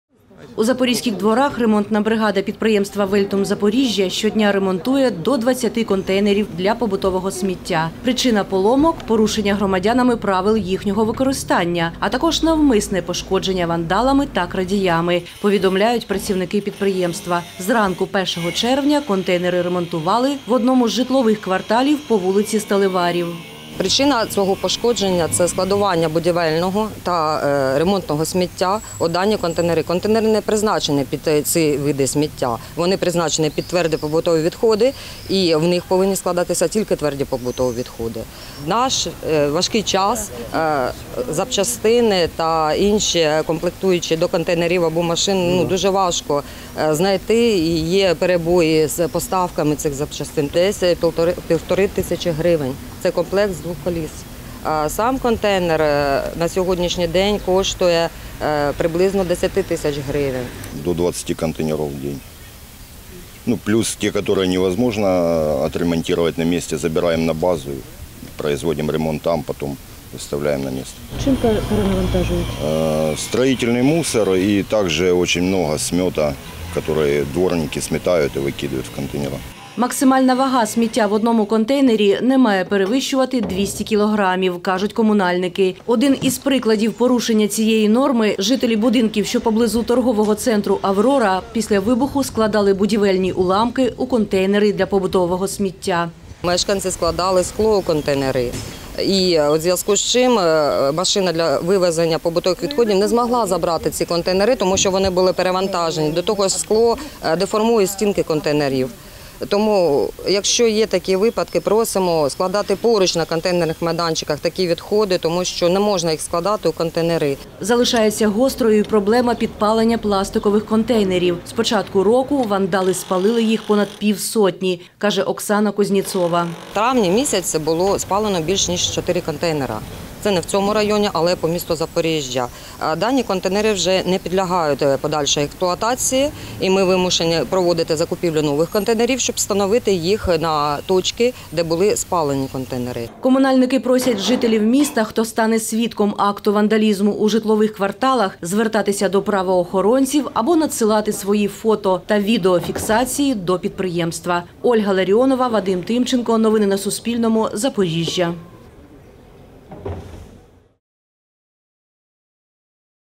У Запоріжжі бригада “Вельтум-Запоріжжя” щодня ремонтує до 20 сміттєвих контейнерів | Новини
У Запорізьких дворах ремонтна бригада підприємства “Вельтум-Запоріжжя” щодня ремонтує до 20 контейнерів для побутового сміття. Причина поломок —  порушення громадянами правил їхнього використання, а також навмисне пошкодження вандалами та крадіями - повідомляють працівники підприємства. Зранку 1 червня контейнери ремонтували в одному з житлових кварталів по вулиці Сталеварів.